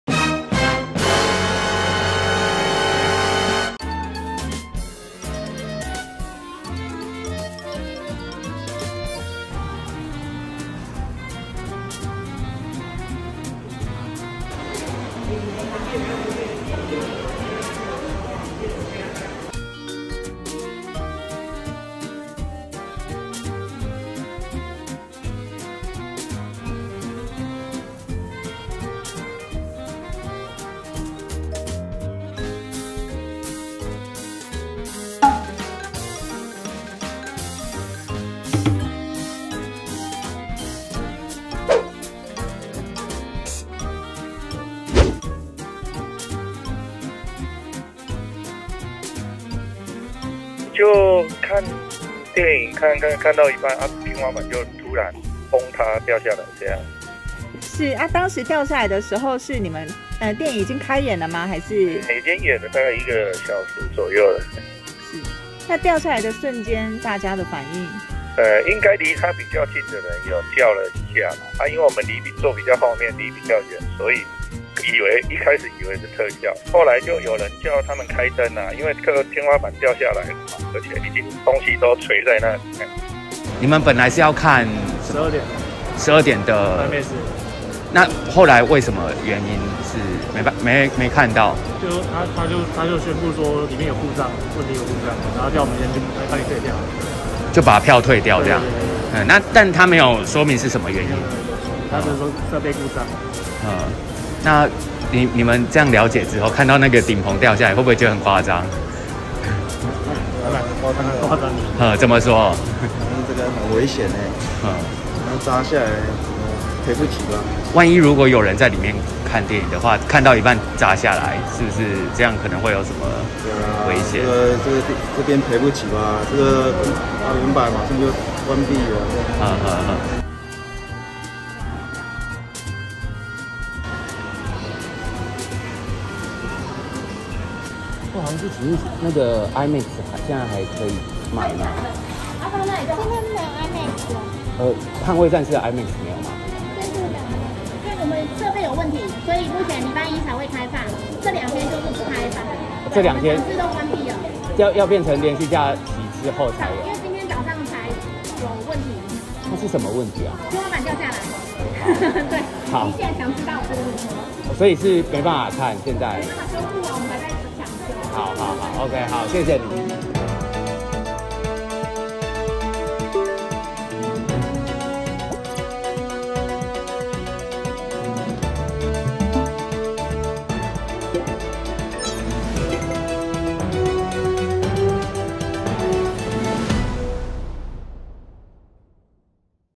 看IMAX「天花板突掉落」 他坐第一排抖爆！│政常發揮
友邦連結區
◎ 訂閱【三立新聞網】最新影片搶先看